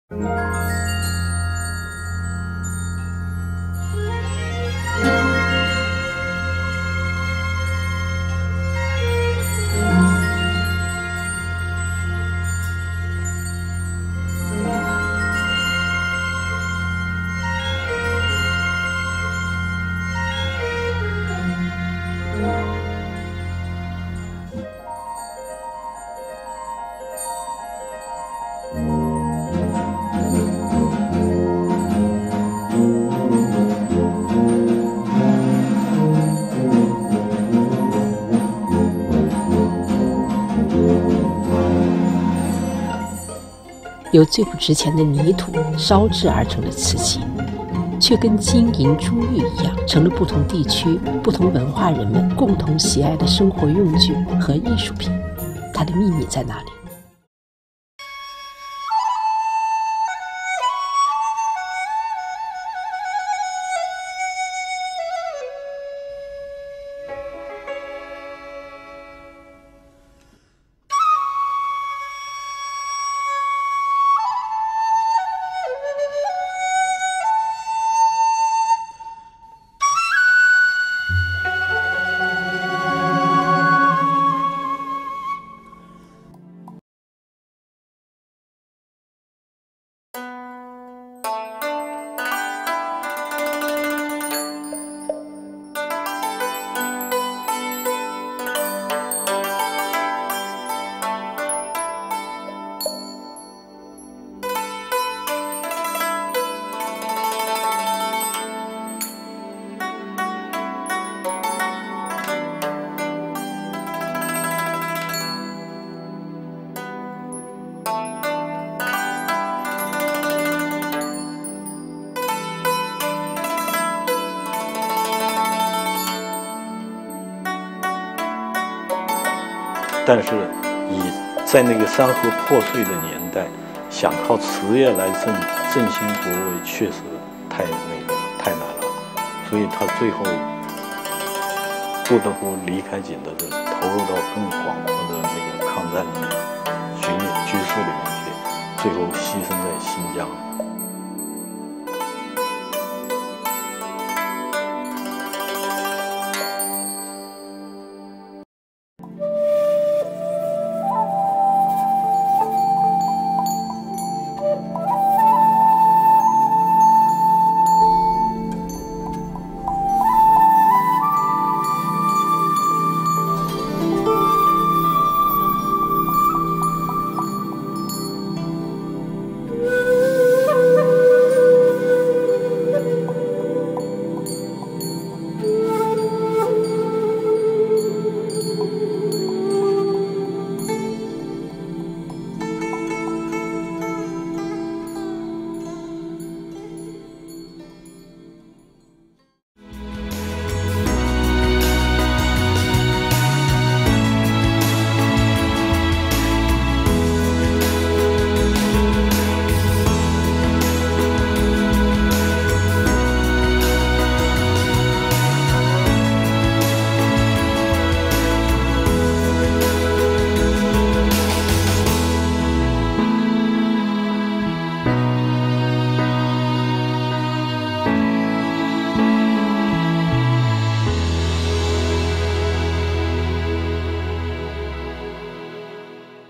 Art of clay & fire: the secret of Chinese porcelain
For thousands of years, porcelain has been crafted from clay and fire. China Album invites you to potter around China's Jingdezhen, the "porcelain capital", to learn more about this delicate earthenware.